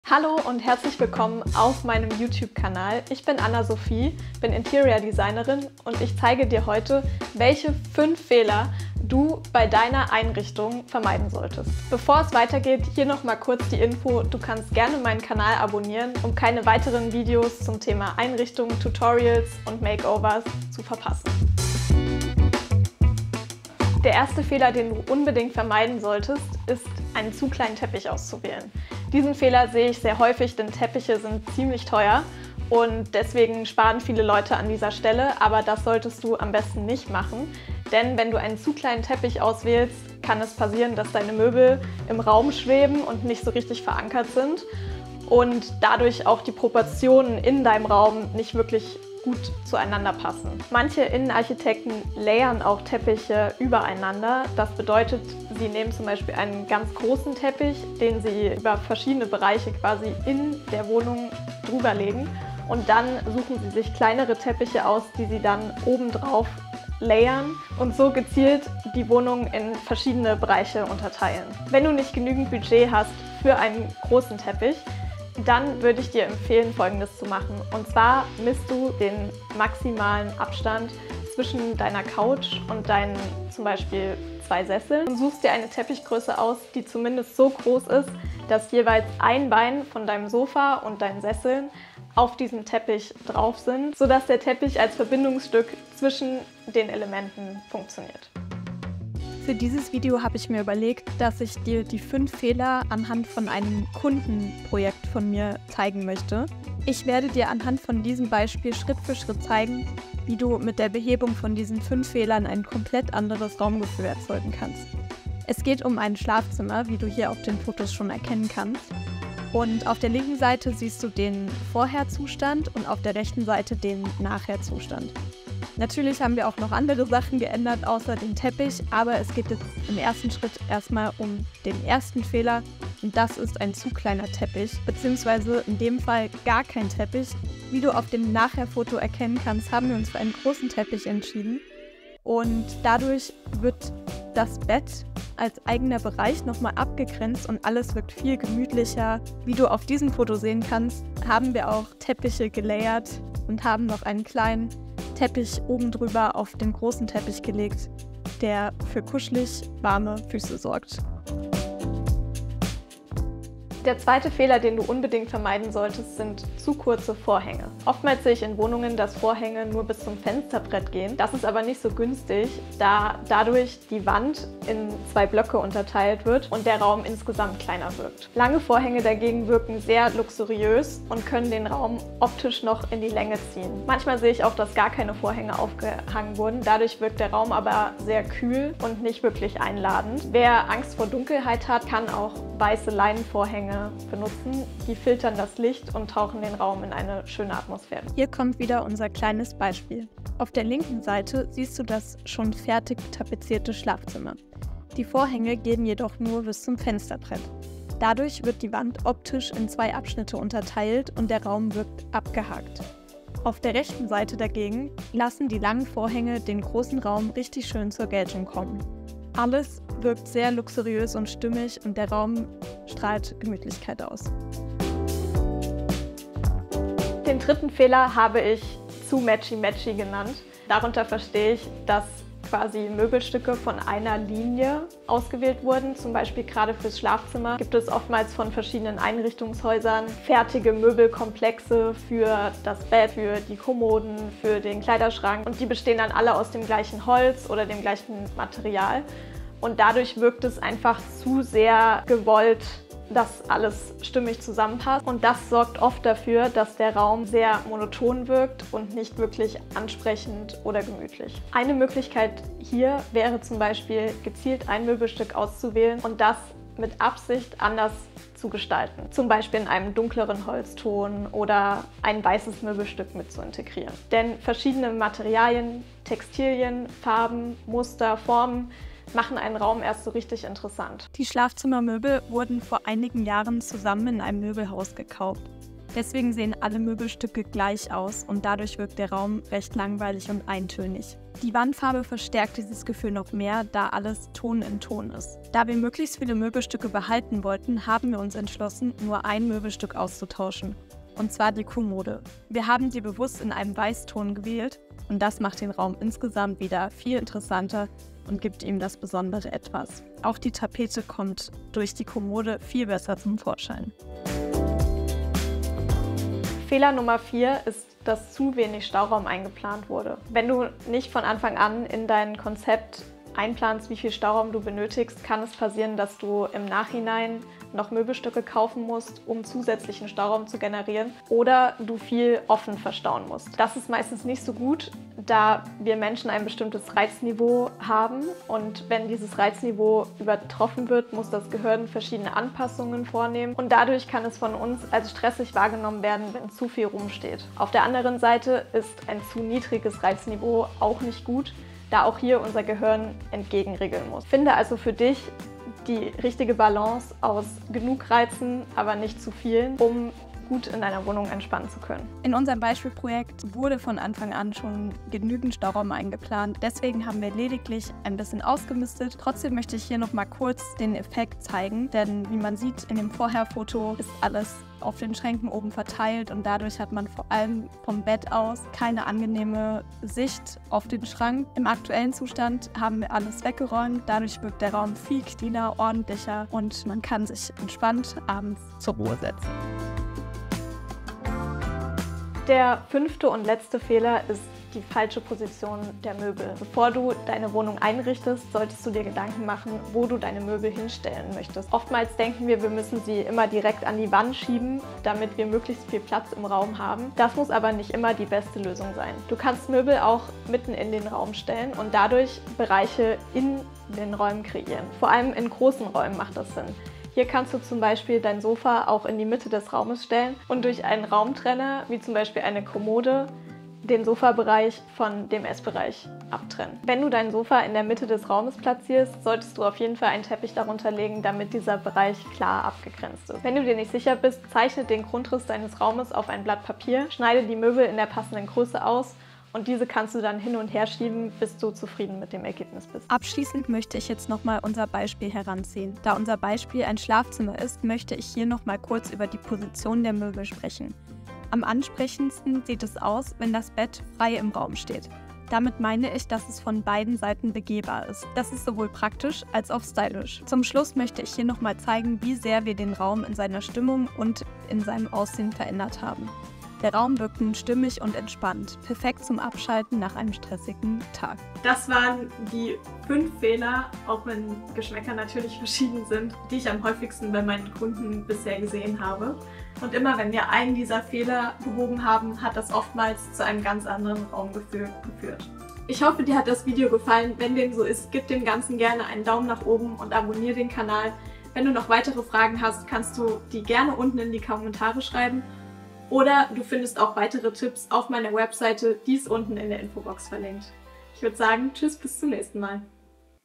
❌5 FEHLER, die du beim EINRICHTEN deiner Räume VERMEIDEN solltest!
In diesem Video erkläre ich dir die 5 häufigsten Fehler, die ich beim Einrichten der Wohnungen und Häuser meiner Kunden sehe. Die Einrichtung spielt eine große Rolle und ich möchte dir ein paar wichtige Tipps und Tricks zeigen, wie du zum Beispiel dein Schlafzimmer oder Wohnzimmer gemütlicher gestalten kannst.